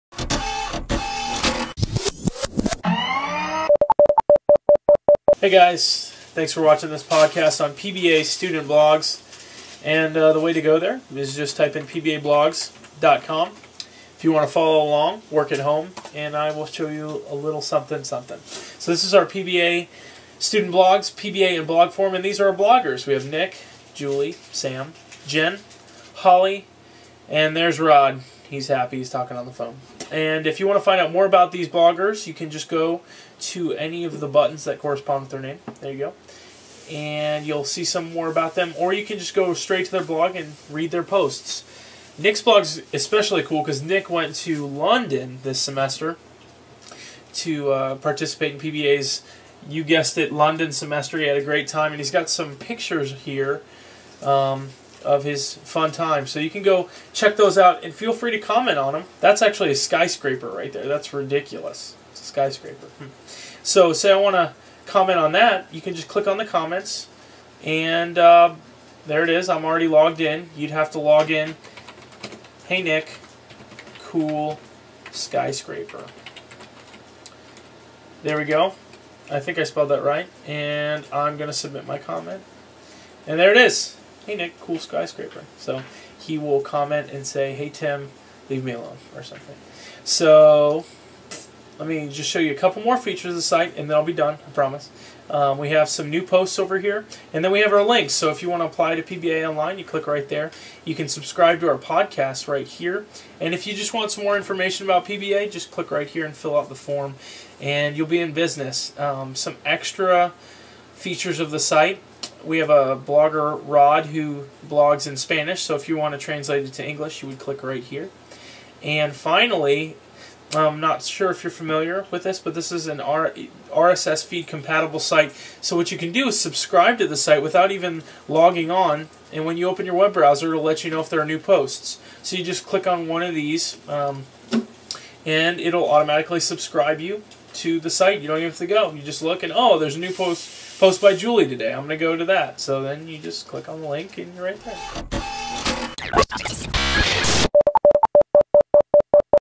On My Computer: Student Blogs
Here's a little how to film about PBA Student Blogs.